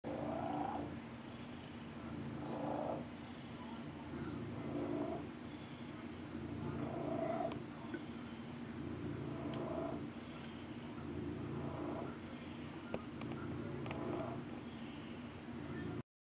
Toilet Larvae
Our careers make it so that we sometimes aren't home for long periods of time. My wife came back to these in a very nasty looking toilet bowl.